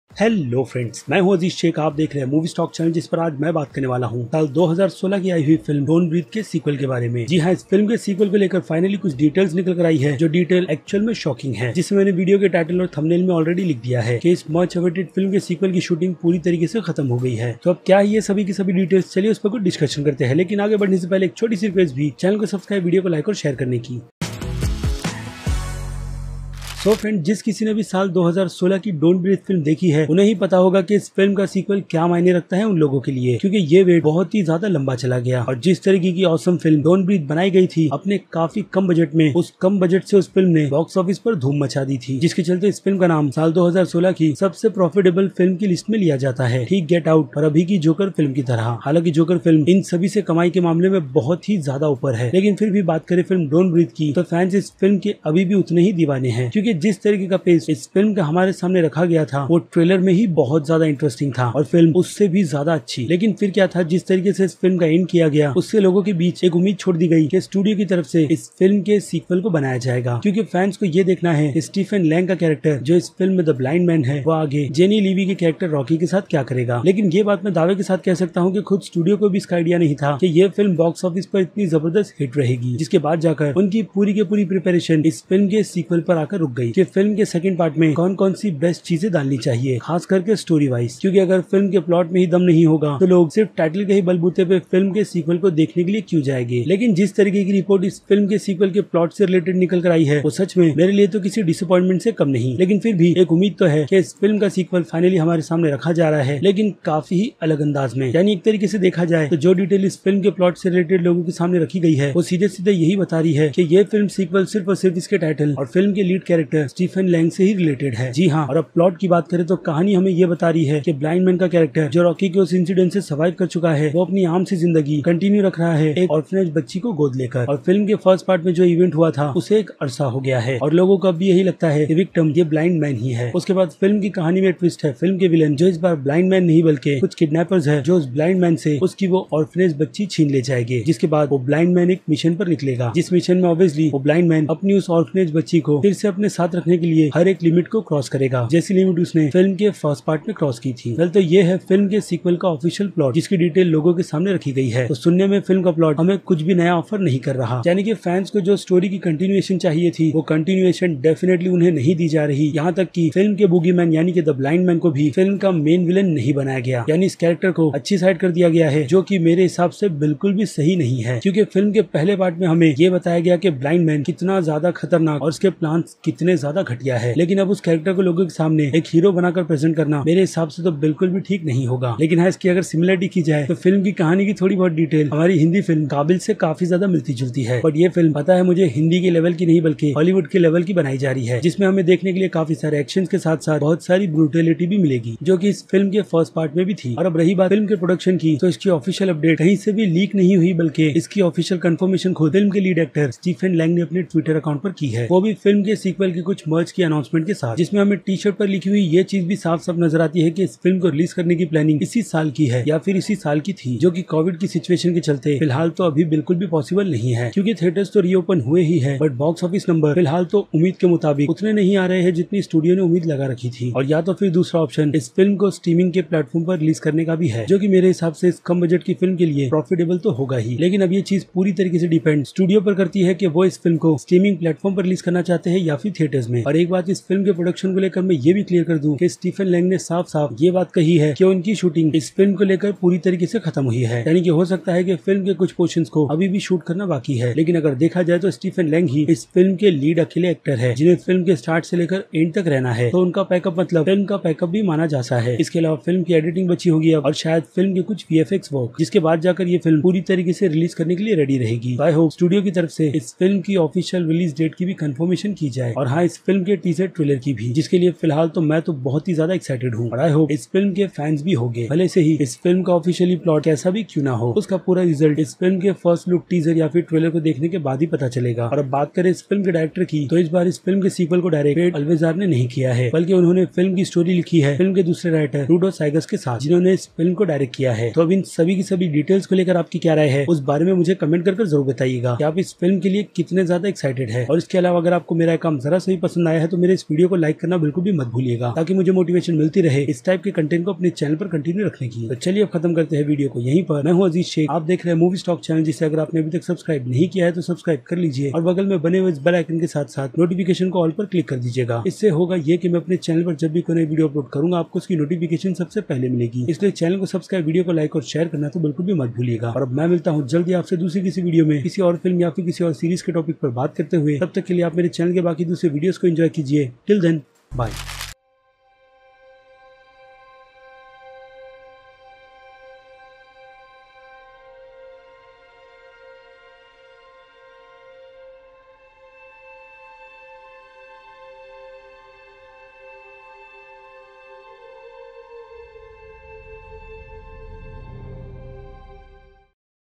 DON’T BREATHE 2 : Movie Shooting Wraps & More Details Breakdown [Explained In Hindi]
Hello friends aaj hum baat karne wale hai don’t breathe 2 film ki kafi sari details ke bare me to kya hai woh chaliye dekhte hai